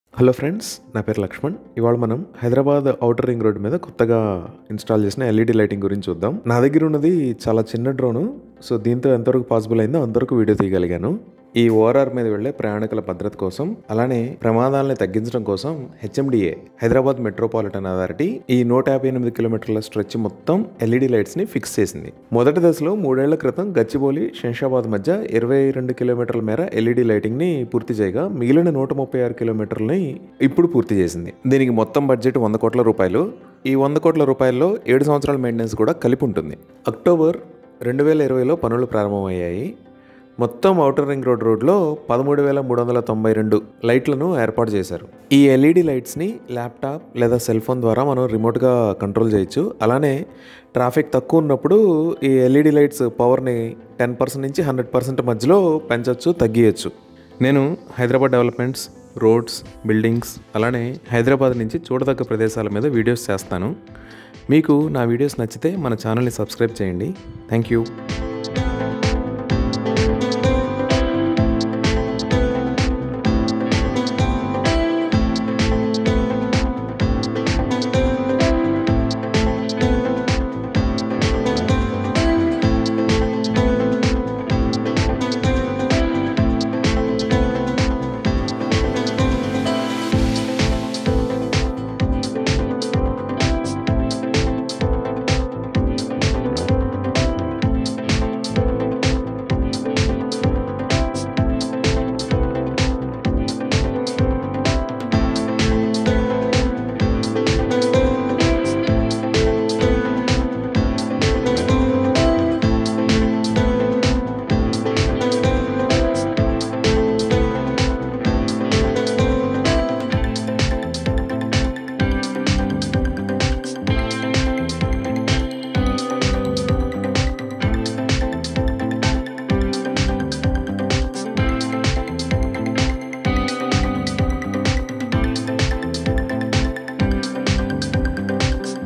LED lighting on ORR │ 13000+ Led lights on Outer Ring Road │
This Video is all about 13000+ LED lights installed on ORR i.e., Outer Ring Road

About LED lighting on ORR:
The LED lighting from Kokapet to Shamshabad on Outer Ring Road (ORR) covering a length of 136 kilometres both the main carriageway, service roads, and junctions with 13,392 lights will be inaugurated by Municipal Administration and Urban Development (MA&UD) Minister KT Rama Rao at Patancheru interchange on Thursday evening. The works were taken up in four packages with a cost of Rs. 100.22 crore which includes an O&M of 7 years with a target to complete in a period of one year. The works were commenced in the month of October 2020 and were completed and ready for commissioning. According to the Hyderabad Metropolitan Development Authority (HMDA) officials, the ORR for a length of 158 km with 8 lanes and service road on either side with 2 lanes was constructed and opened to traffic during the year 2010. The limited lighting was provided initially only for a length of 1km on either side at some of the Interchanges with the conventional lighting system. The lighting of the main carriageway, service roads, and interchanges between Gachibowli to Shamshabad for the length of 22 km and 4 interchanges were taken up and completed in the month of March 2018 with the cost of Rs.30.0 crore which also includes five years of maintenance.